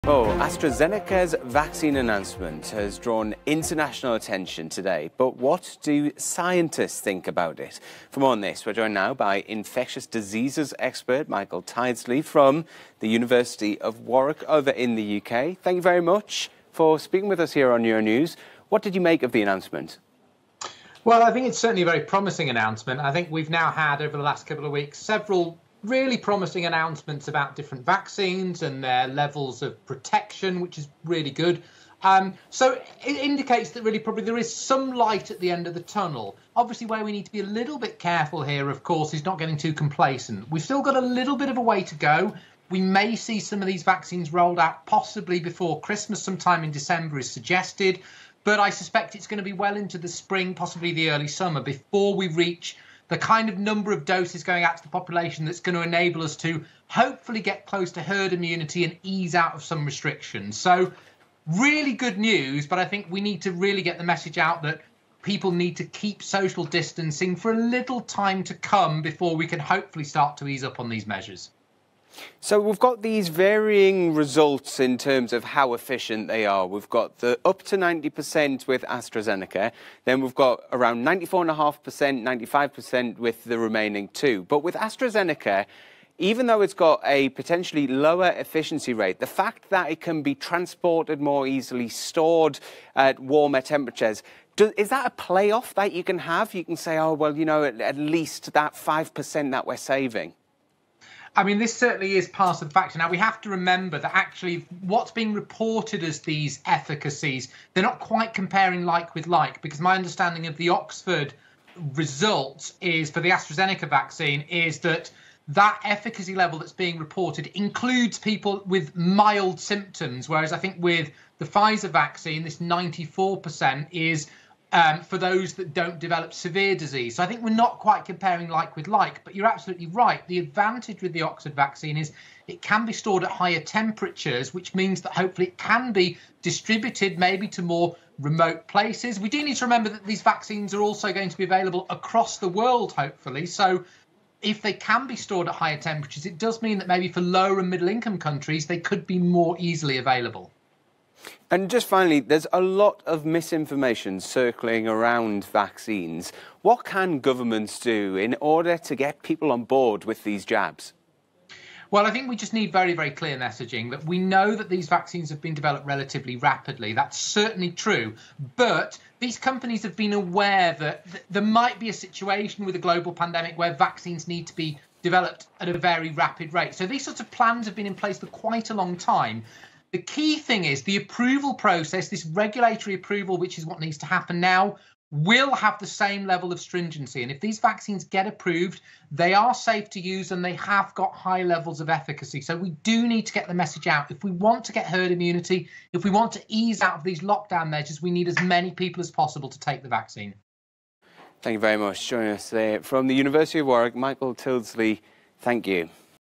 Astrazeneca -Oxford vaccine: Drugs company promises to make 700 milliom active doses by April
AstraZeneca's vaccine announcement has drawn international attention  today. But what do scientists think? For more on the shot we're joined by infectious diseases expert Michael Tildesley from the University of Warwick.